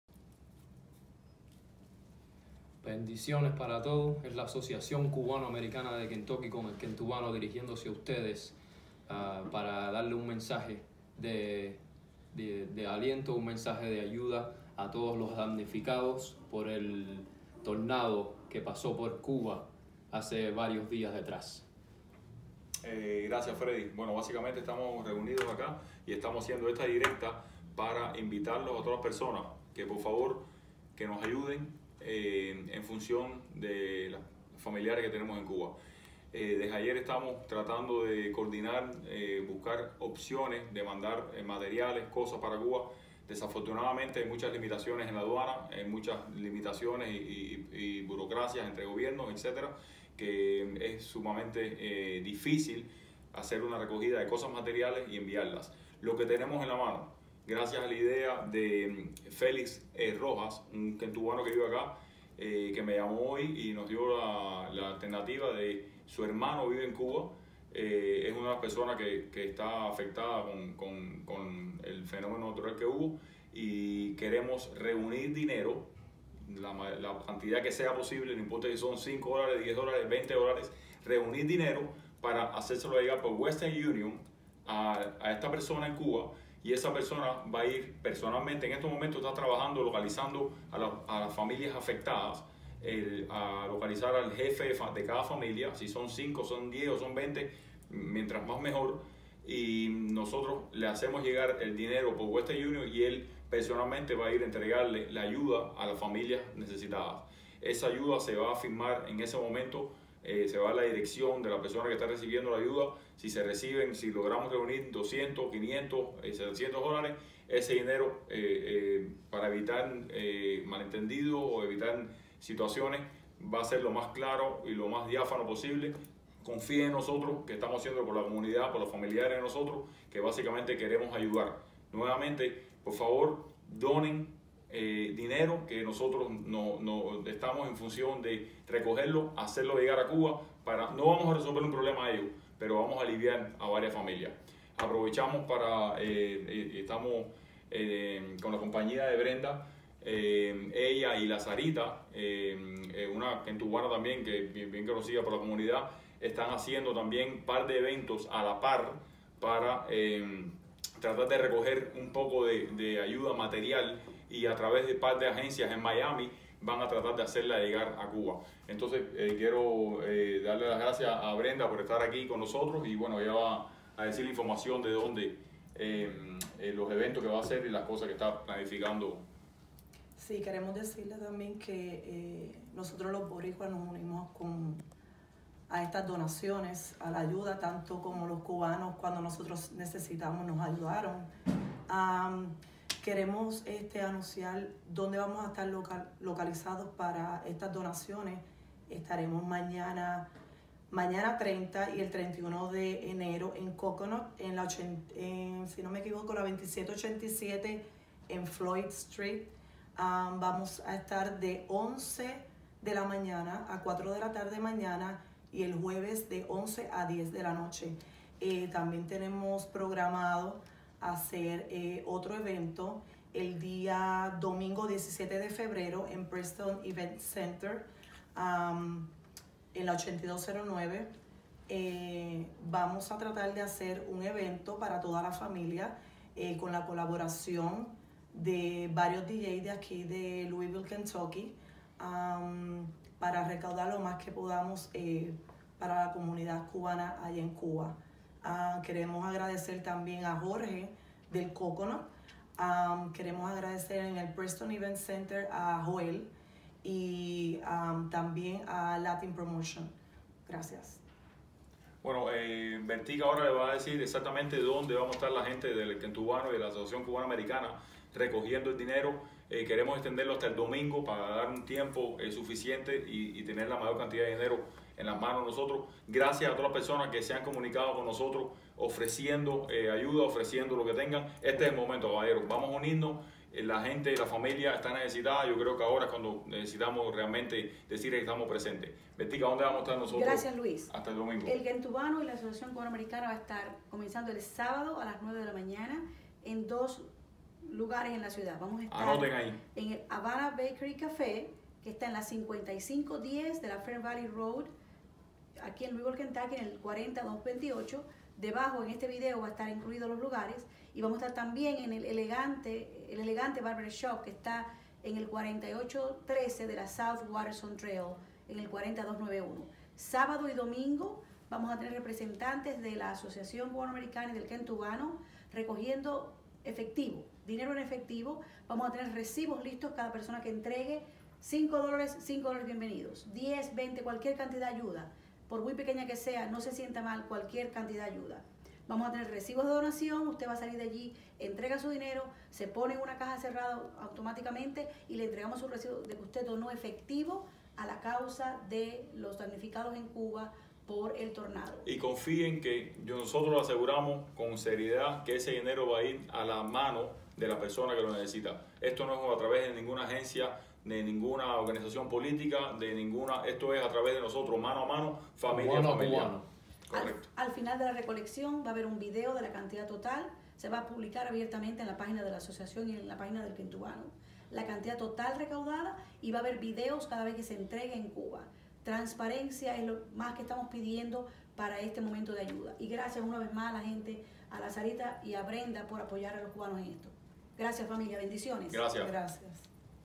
El Kentubano ACAK & Damnificados por tornado
El Kentubano y la Asociación Cubano Americana de Kentucky (ACAK) convocan a recoger dinero para ser entregado personalmente a familias más vulnerables que fueron afectadas por el tornado en La Habana